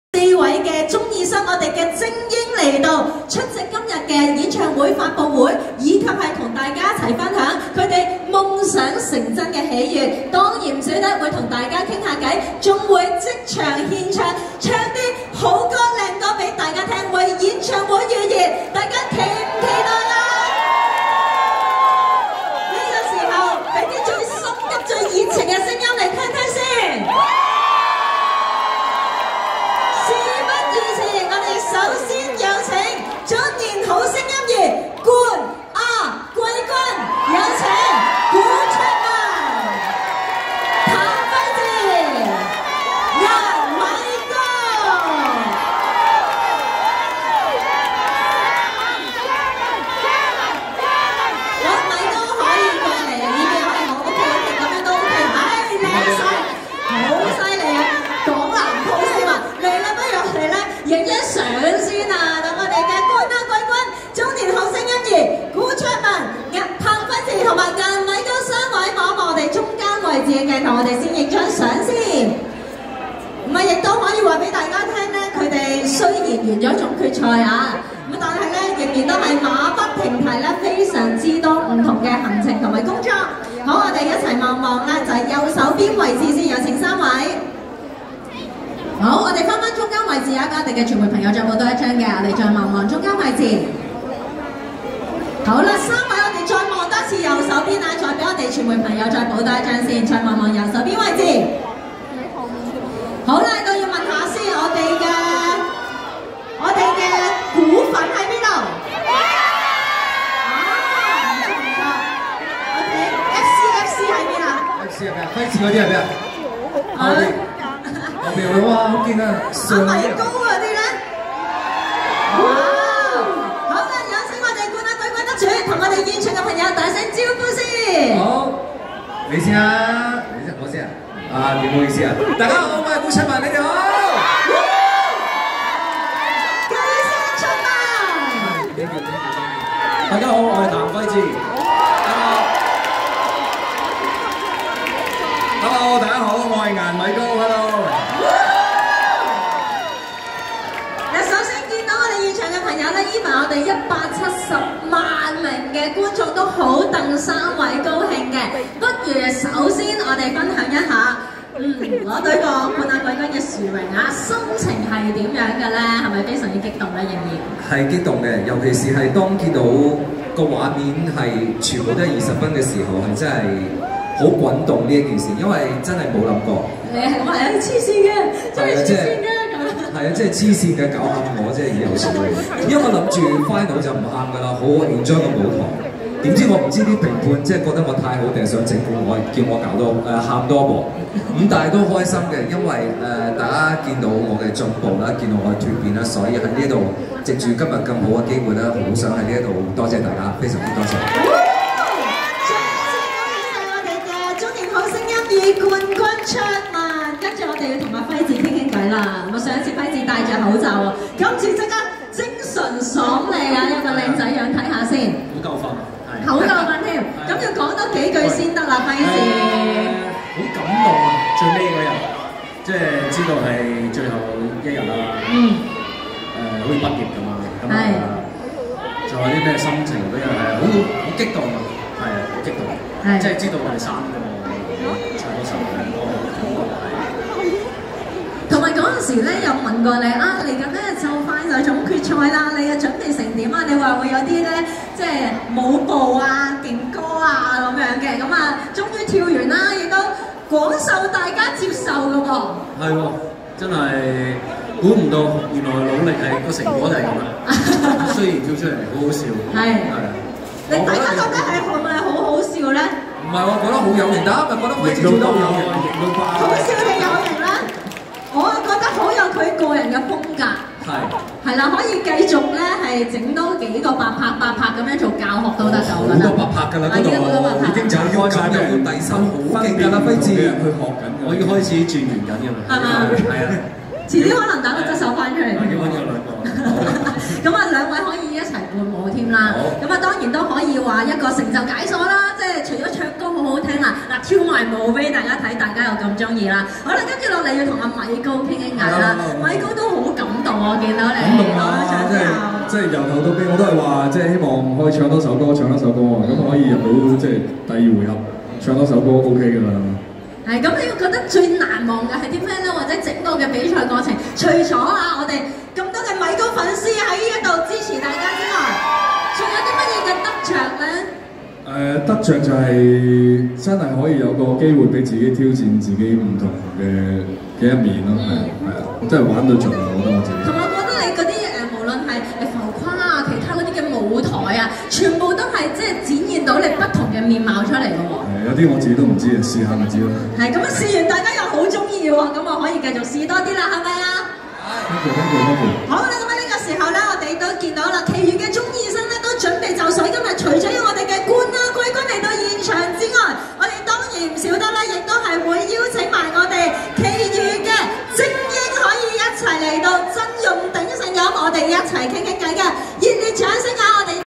中年好聲音2三甲 古淖文 譚輝智 顏米羔 訪問環節@ 中年好聲音2隆重登場見面會
中年好聲音2隆重登場見面會：
日期：2024 年 3 月 29 日 (星期五)
活動時間：下午 2 時
地點：將軍澳中心 G/F 中庭
嘉賓：譚輝智、 顏米羔、 陳俞霏、 劉威煌、 安雅希、 黃劍文、 沈宗賢、 古淖文、 林若盈、 陳蒨葶、 陳燕娜、 王玥、 劉可、